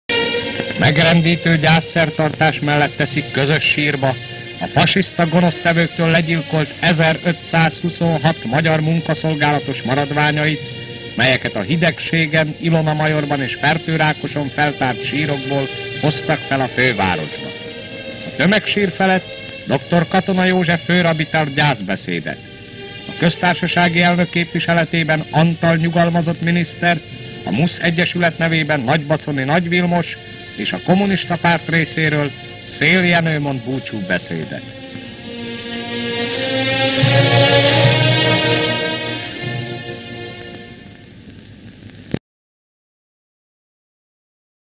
Fertőrákos Anno - 1526 munkaszolgálatos újratemetése
74 évvel ezelőtt, 1947. decemberében temették el a munkaszolgálatban áldozatul esett fertőrákosi testét. A korszak akkor aktuális politikai ideológiája hagy kivetni valót, de erről később írunk. Az áldozatok nyugodjanak békében.

Megrendítő gyászszertartás mellett teszik közös sírba a fasiszta gonosztevőktől legyilkolt 1526 magyar munkaszolgálatos maradványait, melyeket a Hidegségben, Ilona-majorban és Fertőrákoson feltárt sírokból hoztak fel a fővárosba. A tömegsír felett Dr. katona József főrabbi tart gyászbeszédet. A köztársasági elnök képviseletében Antall, nyugalmazott miniszter, a MUSZ Egyesület nevében nagybaconi Nagy Vilmos és a Kommunista Párt részéről Szél Jenő mond búcsúbeszédet.

Hatalmas tömeg a rákoskeresztúri temetőben. Koporsók egymáson. Munkaszolgálatosok gyászszertartása. Dr. Katona József főrabbi beszél. Antall József nyugalmazott miniszter, Nagybaconi Nagy Vilmos, Szél Jenő beszél. Gyászoló tömeg. A koporsókat tömegsírba helyezik.